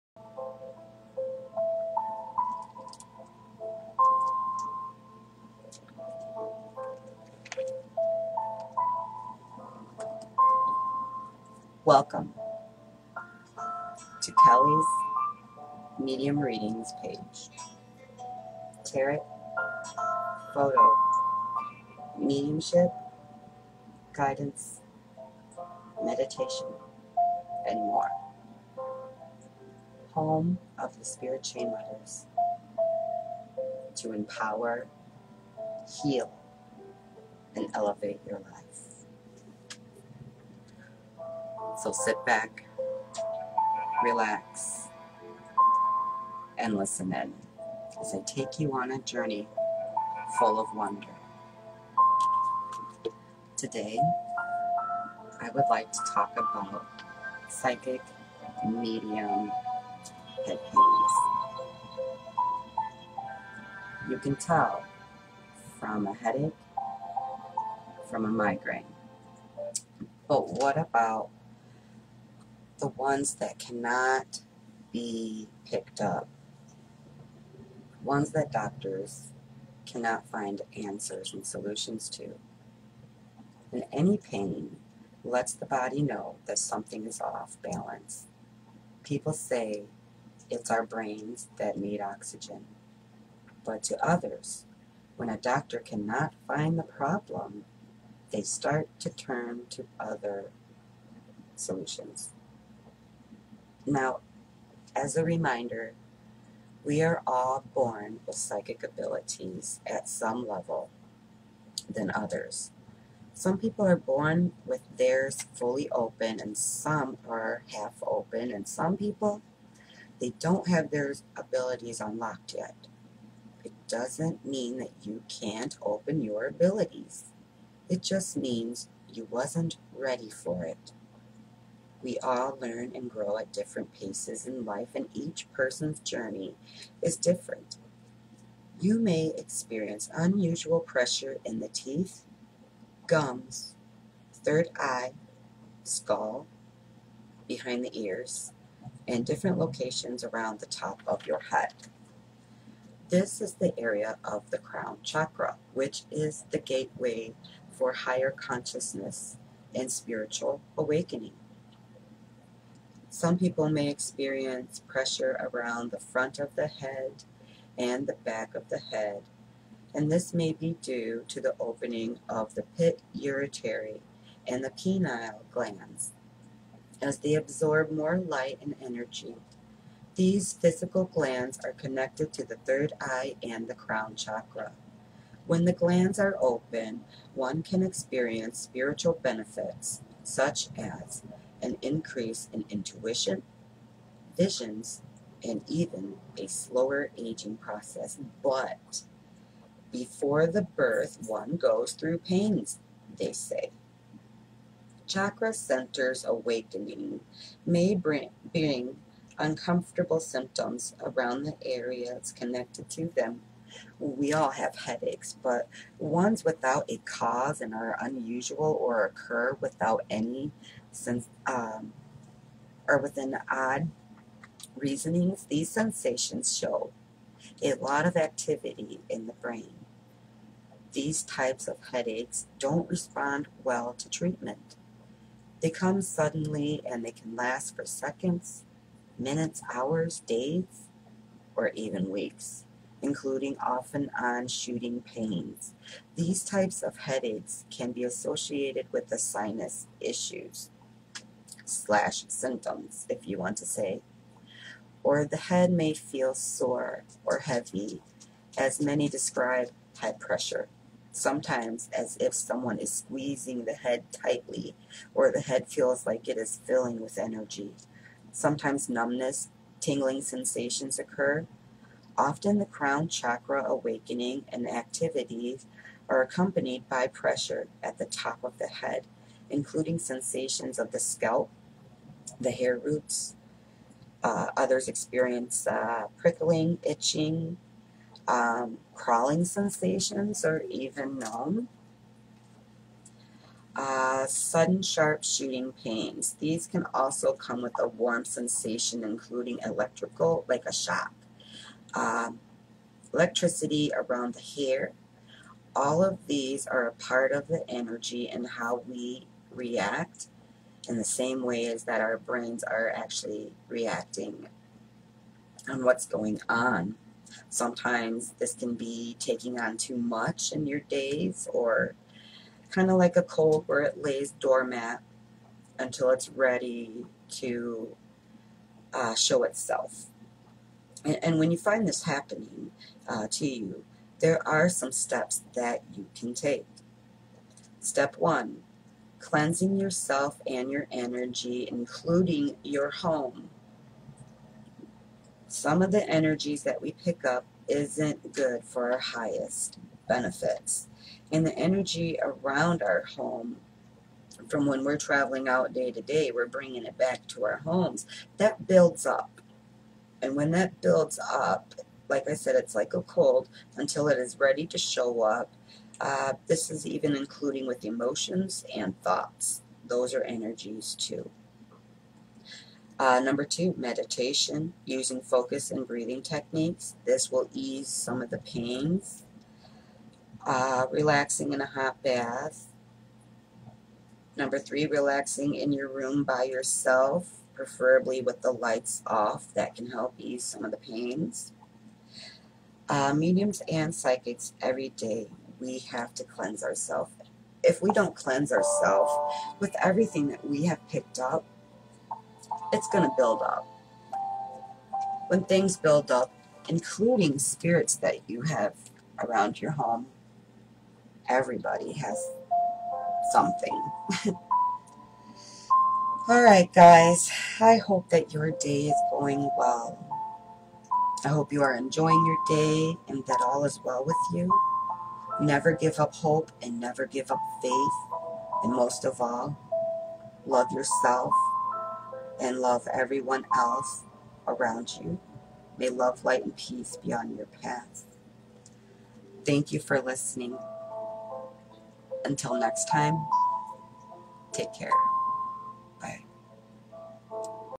Kellys channel
psychic`s , medium`s and head pains ?

here is my thoughts.

love light and peace be with you.
fb readings from other realms